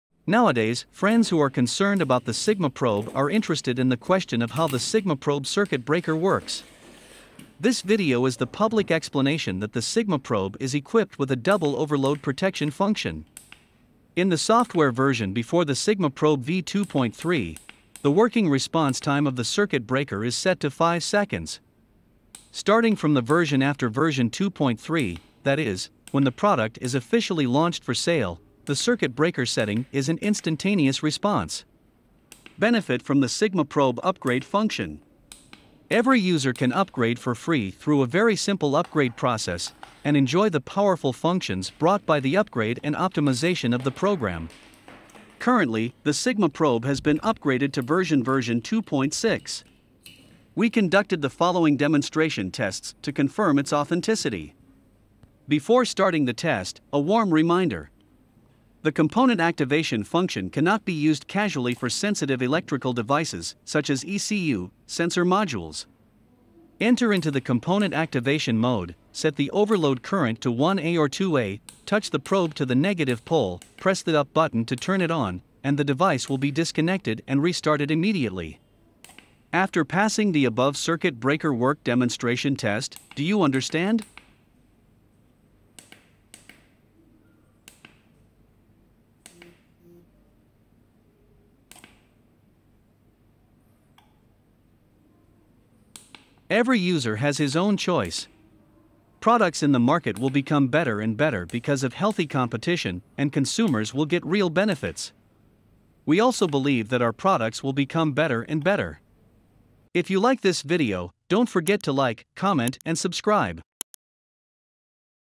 The SigmaProbe circuit breaker function test
Nowadays, friends who are concerned about the SigmaProbe are interested in the question of how the Sigmaprobe circuit breaker works.

This video is the public explanation that the Sigmaprobe is equipped with a double overload protection function.

We conducted the following demonstration tests to confirm its authenticity.